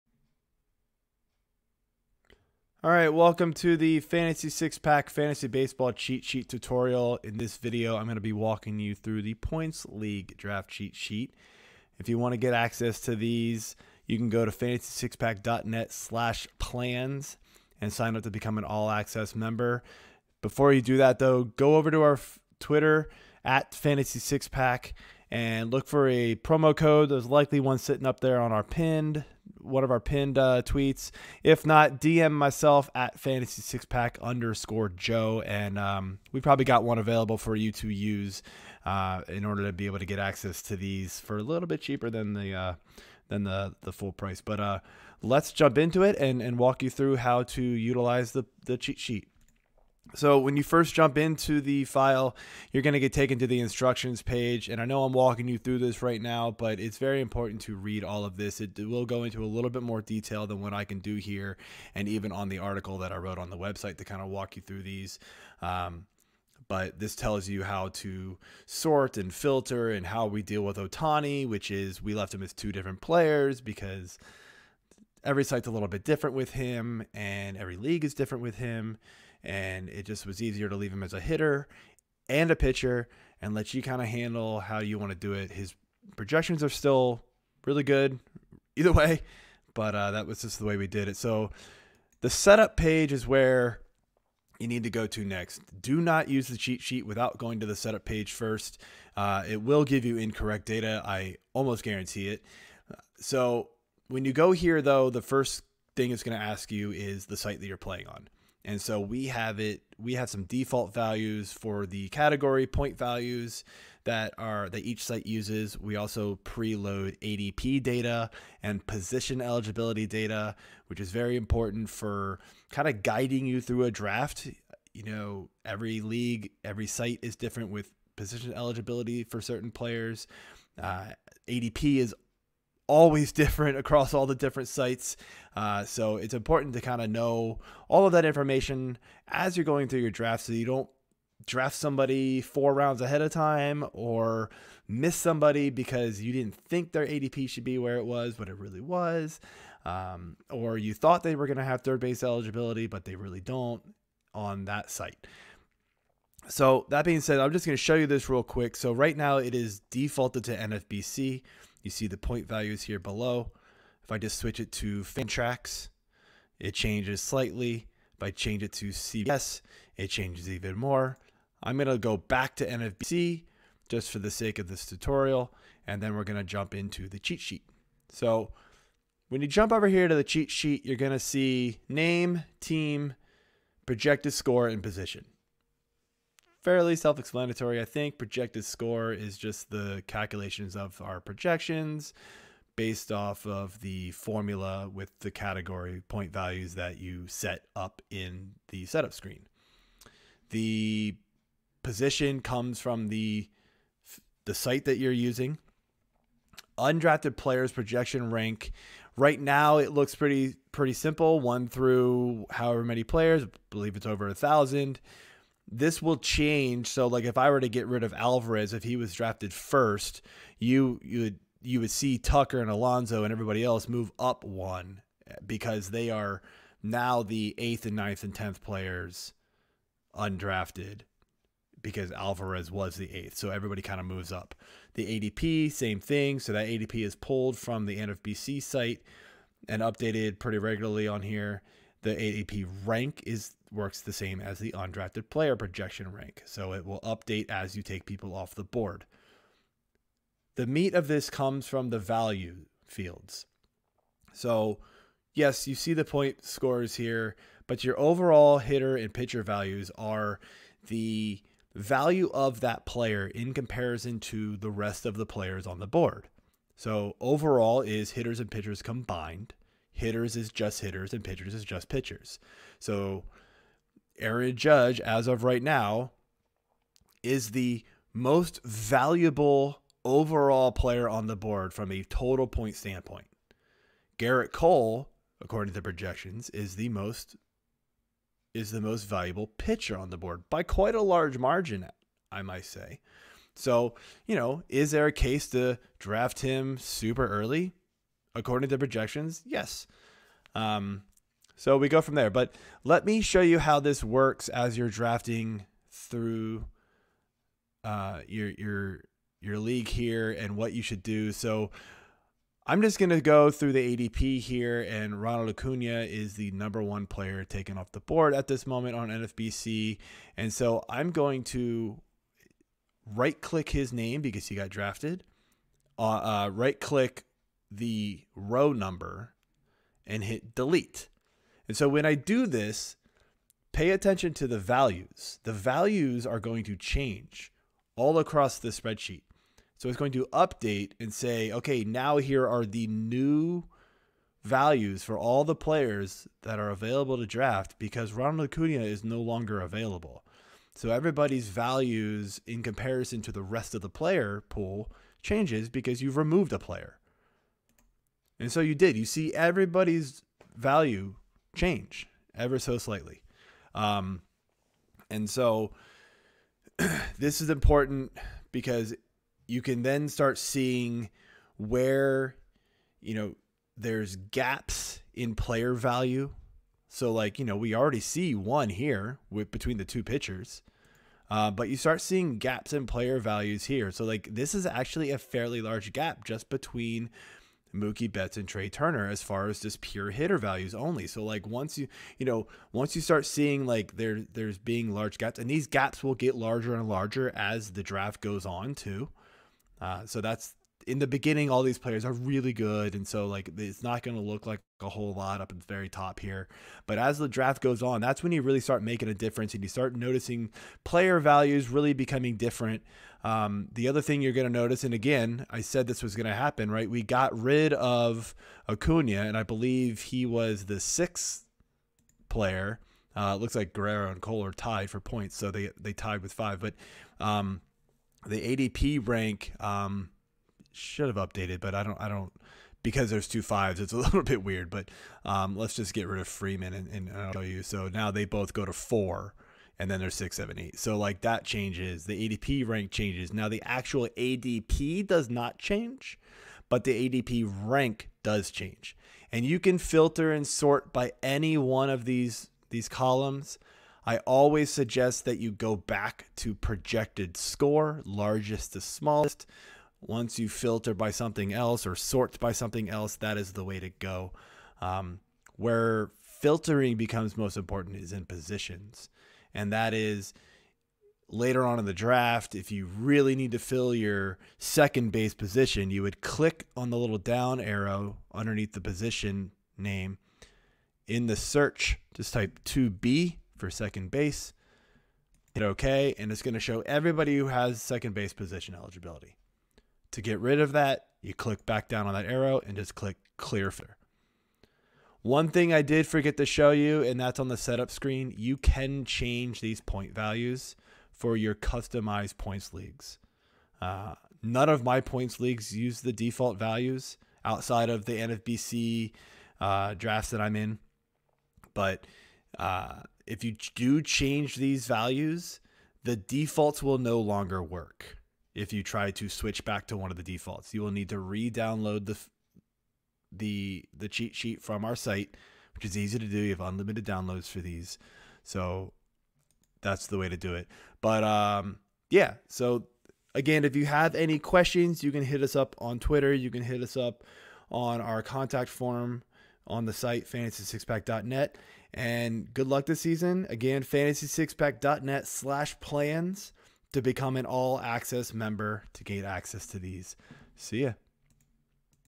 Fantasy Baseball Points League Draft Cheat Sheet Tutorial | Fantasy Six Pack
In this video, I'm going to be giving the Fantasy Baseball Points League Draft Cheat Sheet Tutorial that you can find on Fantasy Six Pack.

I will walk you through how to set it up all the possible customizations, and how to use/read it during your draft.

There are site preset values for points, ADP and Positions for the following sites
- Underdog
- ESPN
- Yahoo
- CBS
- NFBC
- Fantrax (Bestball and Commished leagues)
- RTSports

To get access to this Cheat Sheet, go and become an All-Access member.